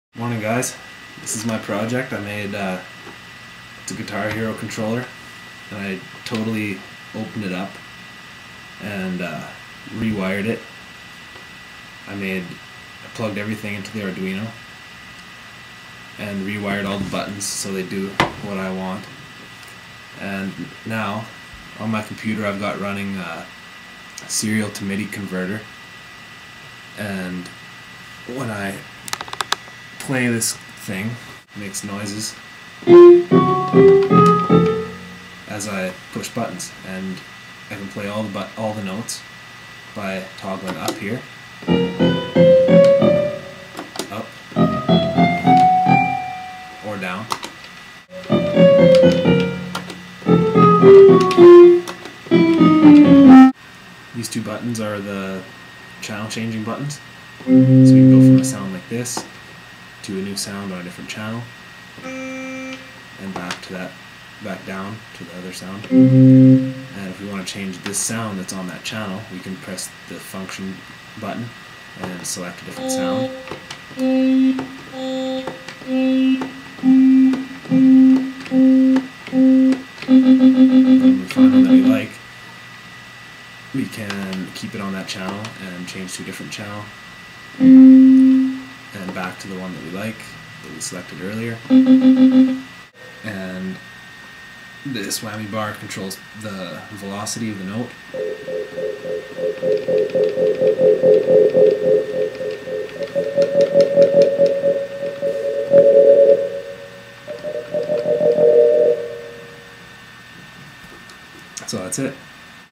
Midi Guitar Hero Controller CS 207 Project
A modified Guitar Hero controller that uses button presses to send different serial commands from an Arduino to my computer where the serial bytes are converted into MIDI sounds. It can play every note(128 notes) and every sound available(128 sounds) in general MIDI. It can use 16 different channels, and each channel can store any of the 128 sounds.

This is a final project for CS 207 class at University of Regina.